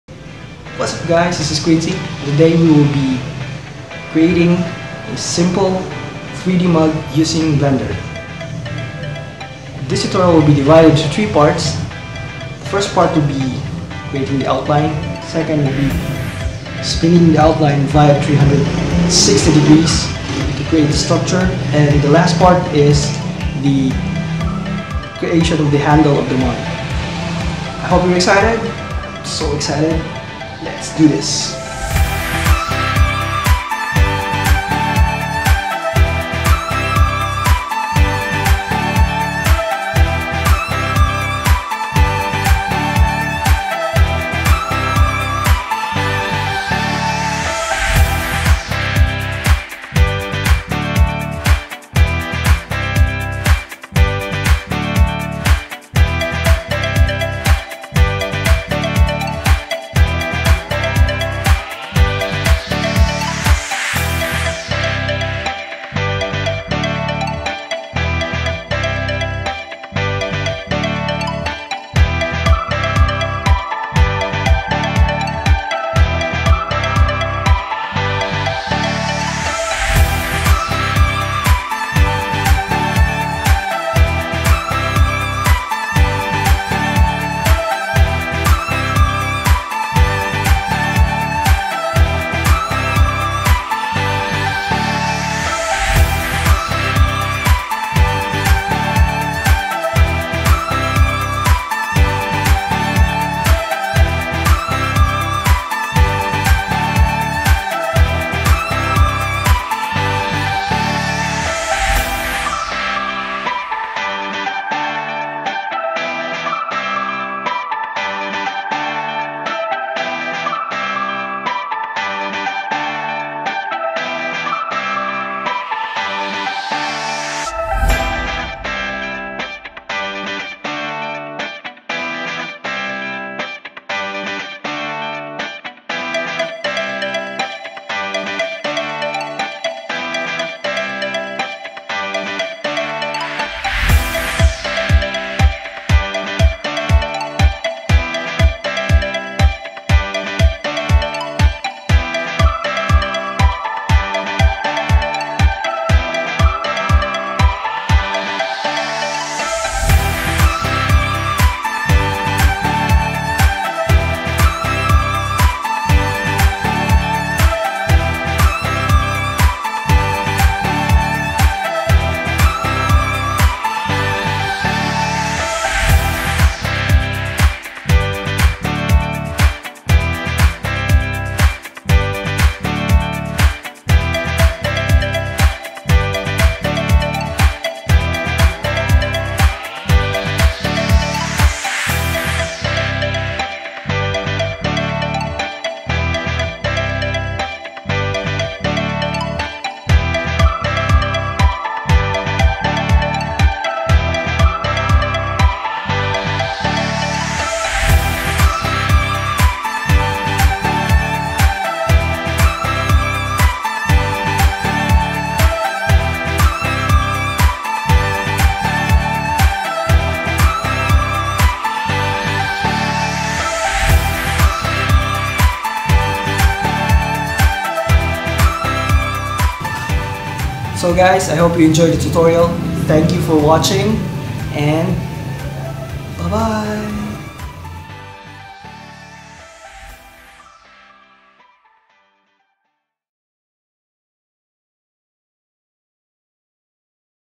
Tutorial On How To Create A 3d Mug In Blender
This tutorial is divided into 3 parts.

1. Creating the half part of the mug (2d) vertices outline
2. Spinning the half part of the mug to create a full 3d model
3. Creating the handle of the mug